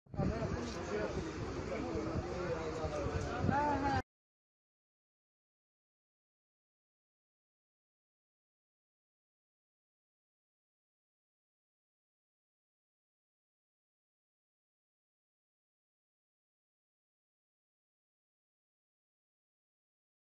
İzmir'de Bir Garip Trafik Kazası
İzmir’in Buca ilçesinde seyir halindeki bir servis aracının sürüklediği otomobil yine hızla bir başka otomobile çarptı. Kazada sürüklenen ikinci otomobilin yarısı, çarpmanın etkisiyle bir evin odasına girdi.